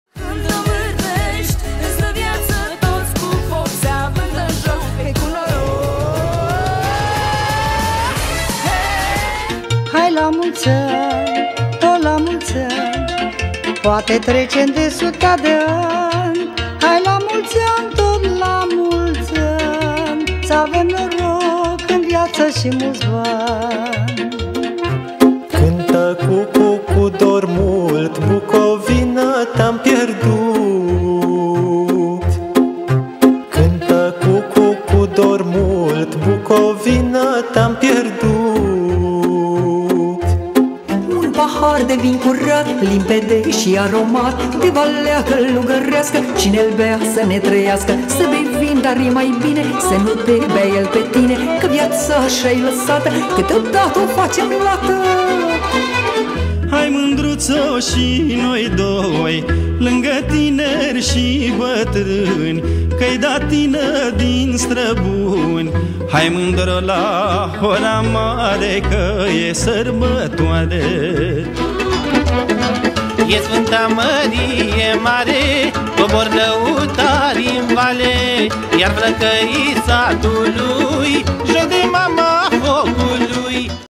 Un spectacol reuşit la Independenţa - partea a III- a
Această ediţie din 29 septembrie este realizată în localitatea Independenţa, judeţul Călăraşi. Artişti binecunoscuţi, din toate zonele folclorice au contribuit din plin la realizarea unui spectacol de excepţie.